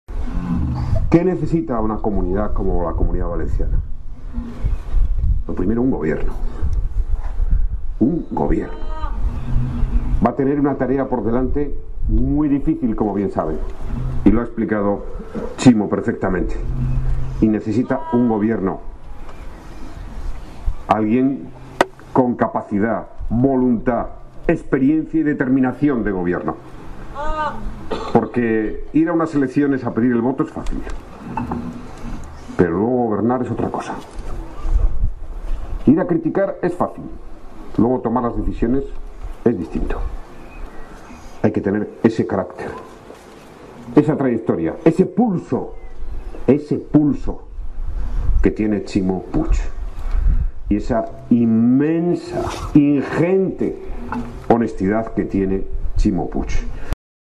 Zapatero arropa en Benicarló las 25 candidaturas socialistas del norte de la provincia
El ex-presidente del Gobierno español, José Luis Rodríguez Zapatero, ha arropado esta tarde la presentación de las candidaturas socialistas de las comarcas castellonenses de Els Ports y Maestrat junto al candidato a la presidencia de la Generalitat Valenciana, Ximo Puig.